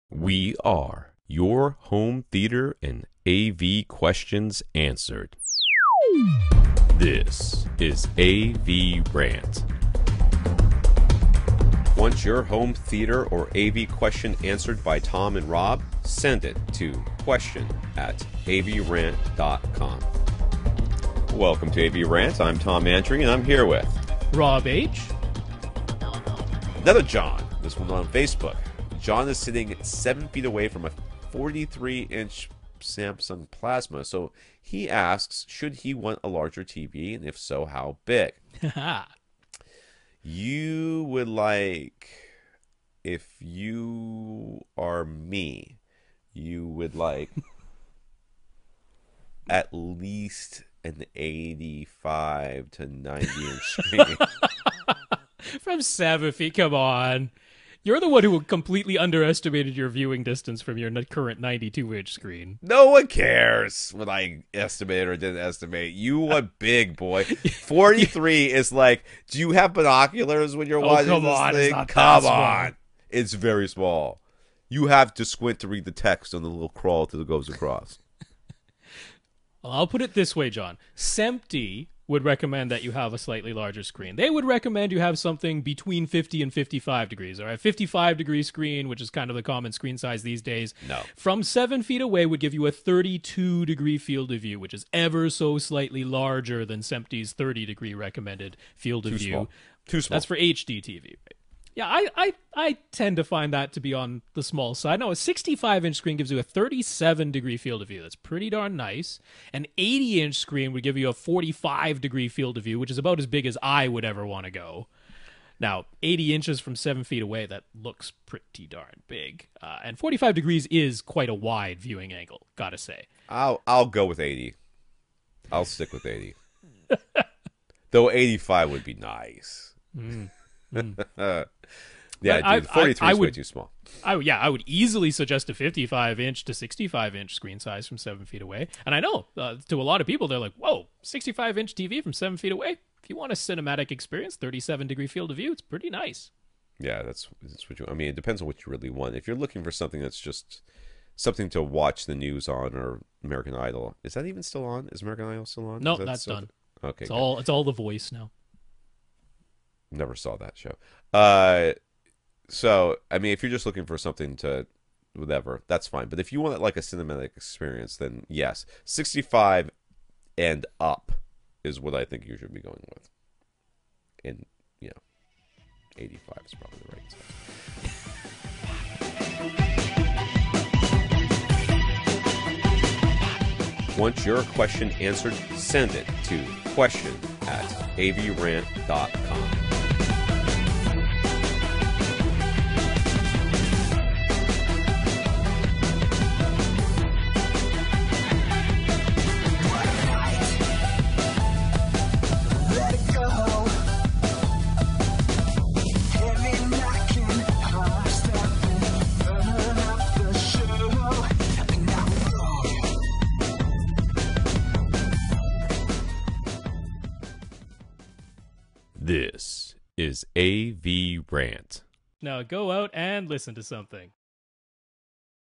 AVRant #538.18 - John - Should I Want A Bigger TV?
John W. asks if he should buy a bigger TV, and if so, how big.

Host: Tom Andry and Rob H.

Art provided by listener Ash

Ending music by listener Ted

Funding for the podcast and videos from YOU!